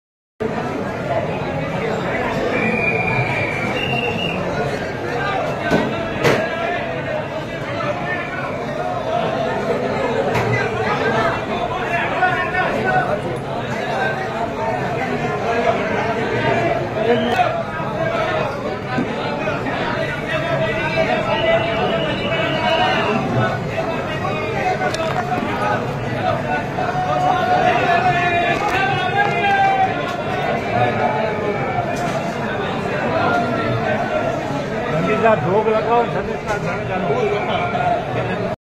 Spirituality Meets Politics: Om Birla's Visit to Ramdevra Samadhi
लोकसभा अध्यक्ष ओम बिरला पहुंचे रामदेवरा बाबा के समाधि पर लगाई ढोक...